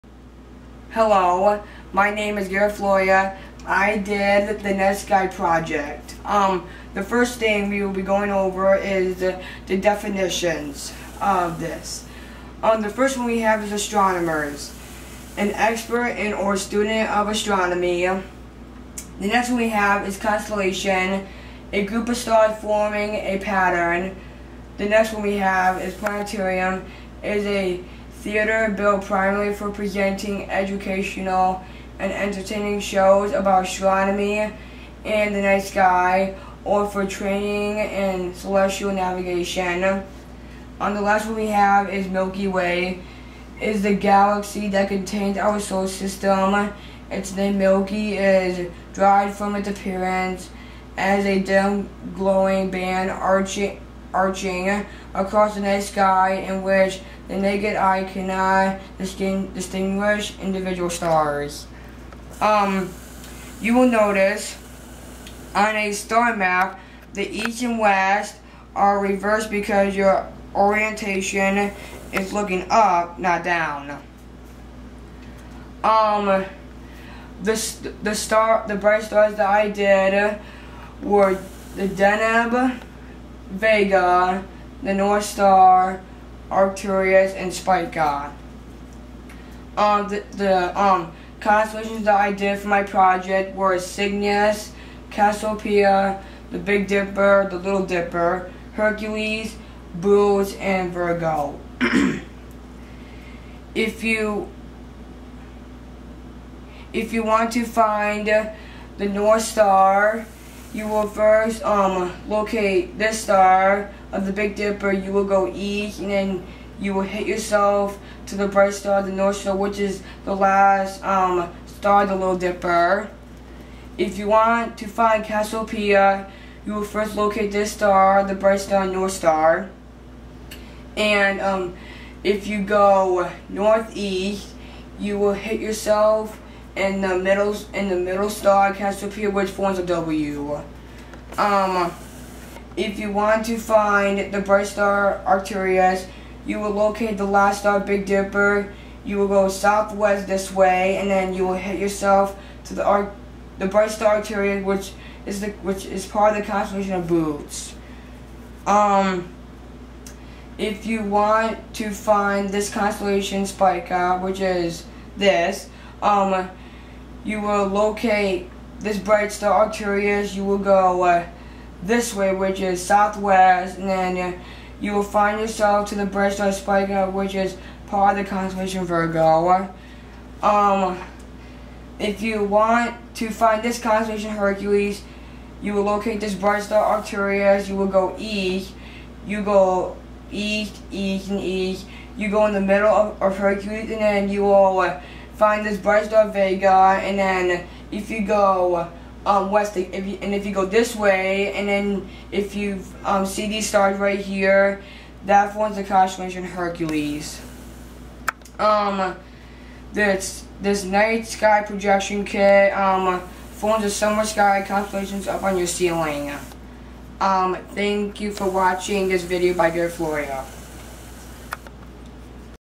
Night Sky Project of Science!
This used to be the very first video I uploaded to YouTube but I deleted it on my personal channel I had but now it's on this channel. This video was filmed on May 28, 2015. Prepare for cringe lollllll